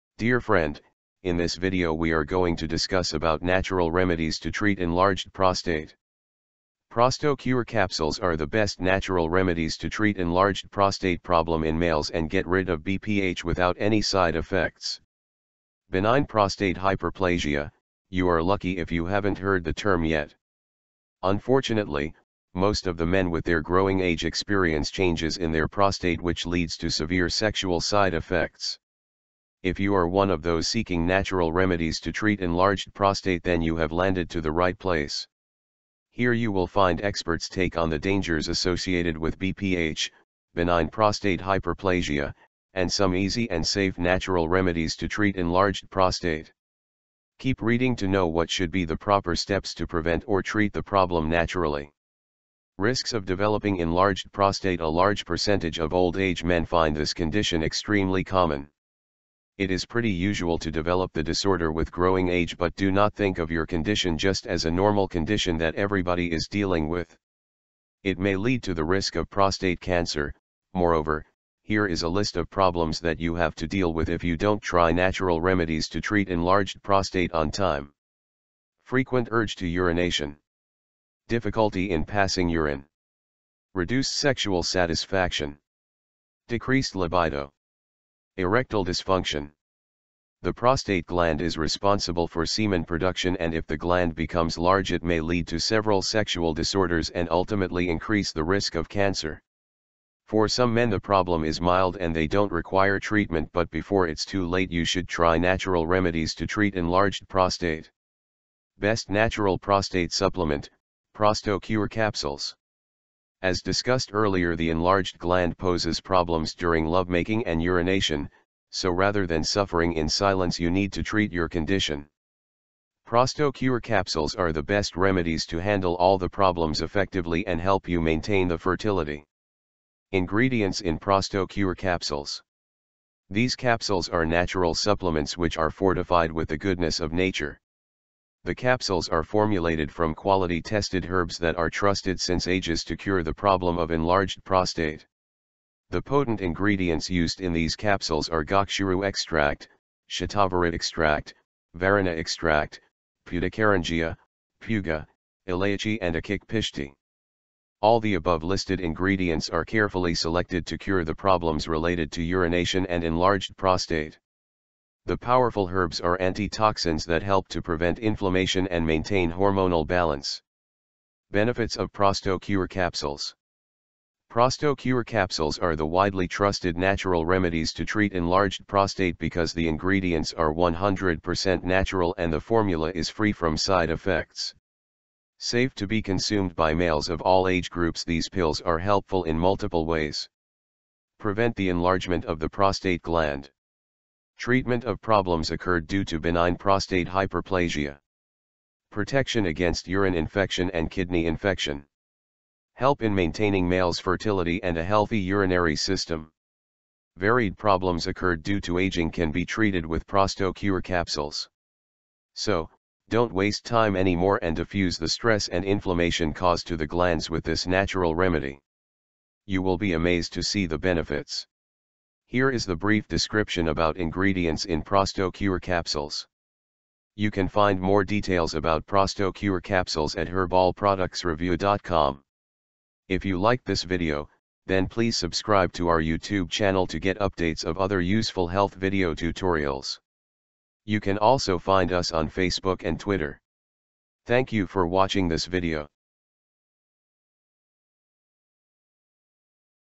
Natural Remedies To Treat Enlarged Prostate Problem In Males
You can find more natural remedies to treat enlarged prostate

Dear friend, in this video we are going to discuss about natural remedies to treat enlarged prostate. Prostocure capsules are the best natural remedies to treat enlarged prostate problem in males and get rid of BPH without any side effects.

Natural Remedies To Treat Enlarged Prostate